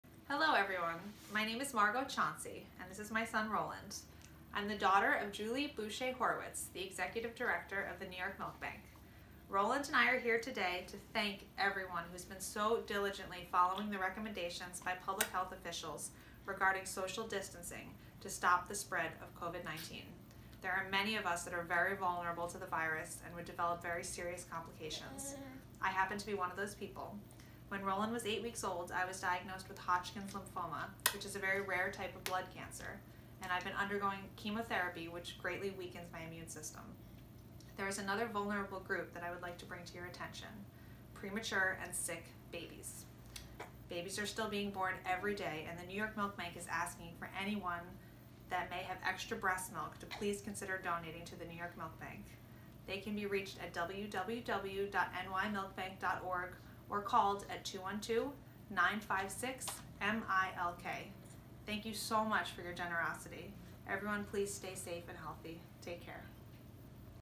Your Extra Breastmilk Can Save A Premature Infant's Life
If you have a surplus of breastmilk and would like to empty your freezer, please fill out a donate milk form on our website, thank you!